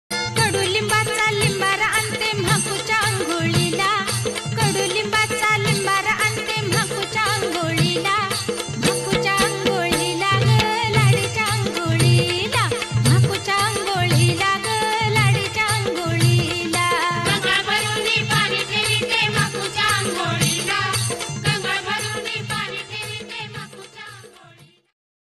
@लाडीच्या आंघोळीला बैल गाडी निघाली / @पावंड किंग खोंडाची जोड
लाडीच्या आंघोळीला बैल गाडी निघाली / पावंड किंग खोंडाची जोड चं चिंचणी यात्रा